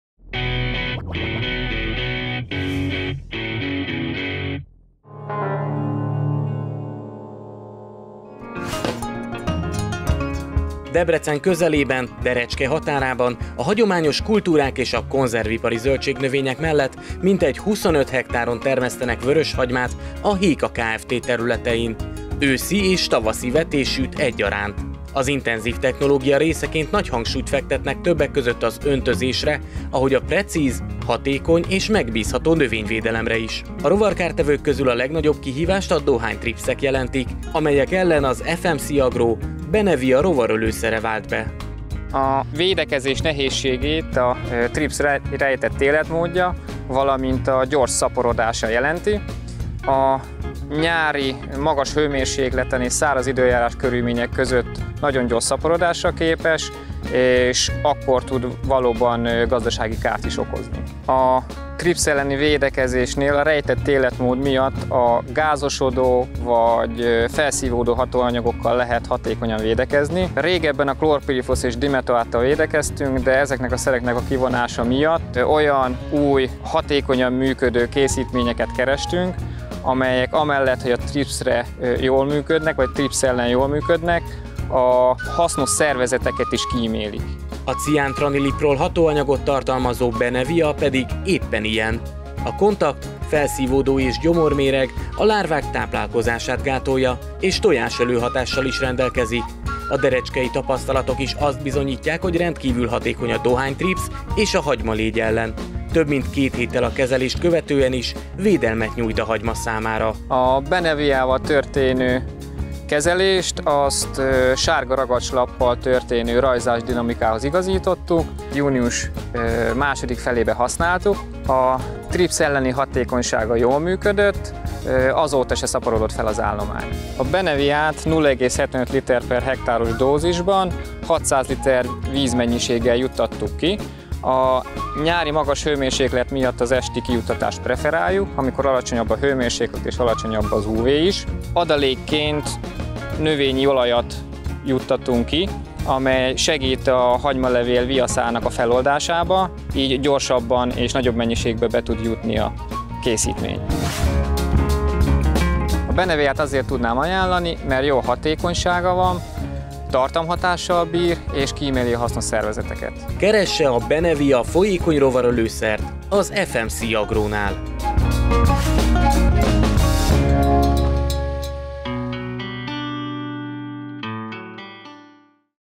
Nem találni tripszeket a derecskei hagymában - Öntözés és növényvédelem a Héka Kft.-nél
Debrecen közelében, Derecske határában a hagyományos kultúrák és a konzervipari zöldségnövények mellett mintegy 25 hektáron termesztenek vöröshagymát a Héka Kft. területein - őszi és tavaszi vetésűt egyaránt. Az intenzív technológia részeként nagy hangsúlyt fektetnek többek között az öntözésre, ahogy a precíz, hatékony és megbízható növényvédelemre is. A rovarkártevők közül a legnagyobb kihívást a dohánytripszek jelentik, amelyek ellen az FMC-Agro Benevia rovarölő szere vált be.

A ciántraniliprol hatóanyagot tartalmazó Benevia pedig éppen ilyen. A kontakt, felszívódó és gyomorméreg a lárvák táplálkozását gátolja, és tojásölő hatással is rendelkezik. A derecskei tapasztalatok is azt bizonyítják, hogy rendkívül hatékony a dohánytripsz és a hagymalégy ellen - több mint két kéttel a kezelést követően is védelmet nyújt a hagyma számára.